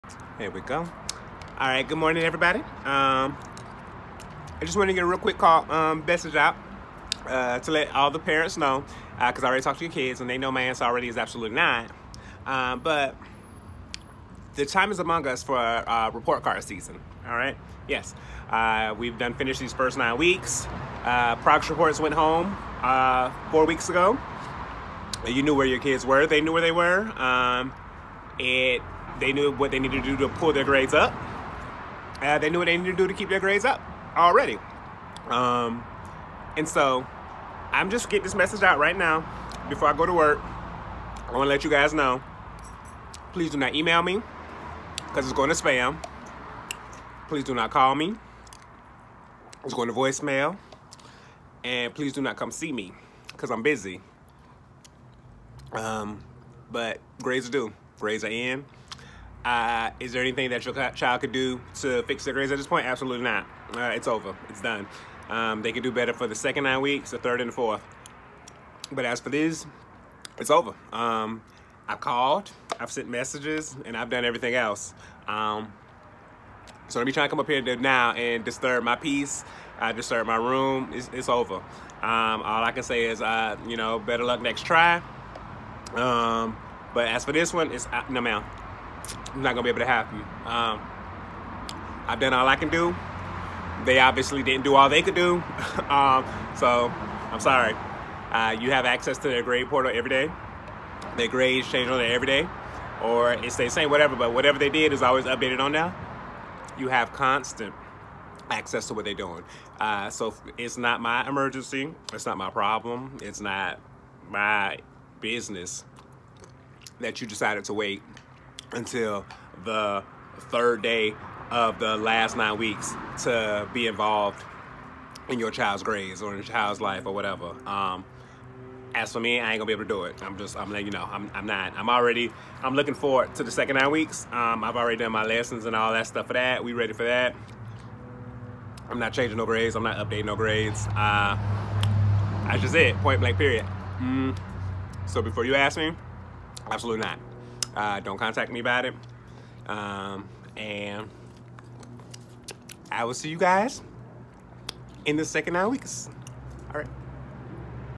It’s report card season and you should already know!
It’s report card season! Understand that it’s over and please don’t get caught up in the wave of no’s! 🤣🚫🚙🤙🏾